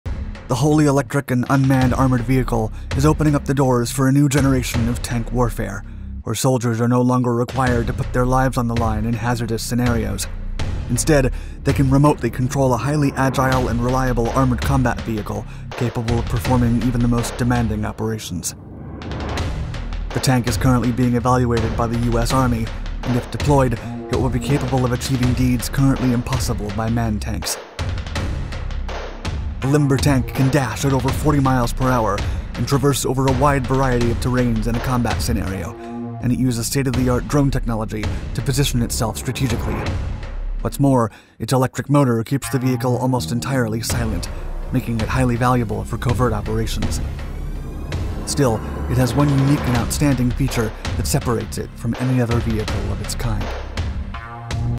Ripsaw M5 Unmanned Super Tank 🔥
Warfare technology has taken a drastic leap forward with the recent unveiling of the Ripsaw M5 Electric Drive Super Tank.

The wholly electric and unmanned armored vehicle is opening up the doors for a new generation of tank warfare, where soldiers are no longer required to put their lives on the line in hazardous scenarios; instead, they can remotely control a highly agile and reliable armored combat vehicle capable of performing even the most demanding operations.

The tank is currently being evaluated by the US army, and if deployed, it will be capable of achieving deeds currently impossible by manned tanks.

The limber tank can dash at over 40 miles per hour and traverse over a wide variety of terrains in a combat scenario, and it uses state-of-the-art drone technology to position itself strategically. What’s more, its electric motor keeps the vehicle almost entirely silent, making it highly valuable for covert operations.

Still, it has one unique and outstanding feature that separates it from any other vehicle of its kind…